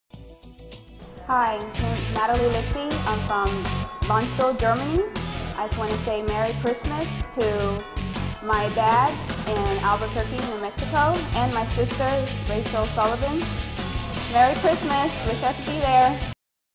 Military Greetings: TSgt Natalie Litsey
Air Force Tech Sgt. Natalie LitseyAirman Reynaldo Gutierrez stationed at Landstuhl, Germany, sends holiday greetings to friends and family in New Mexico.